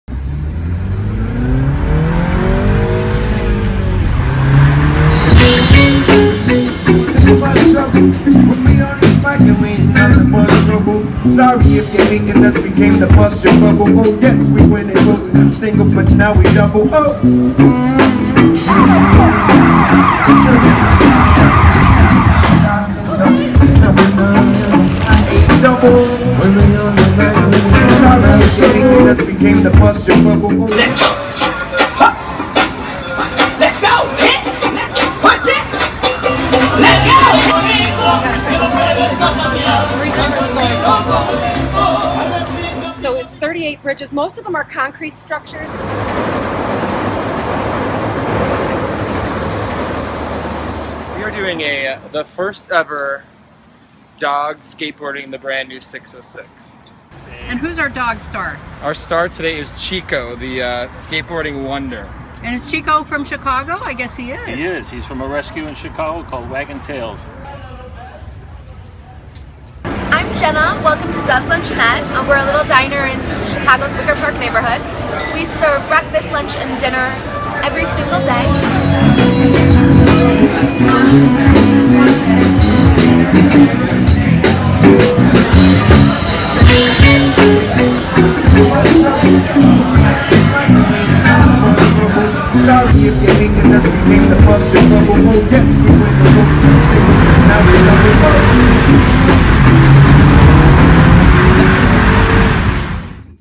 Slice of Chicago 3
Parks, food, neighbourhoods, gardens, skateboarding dogs, The 606, architecture, so much to do in Chicago, I have to go back.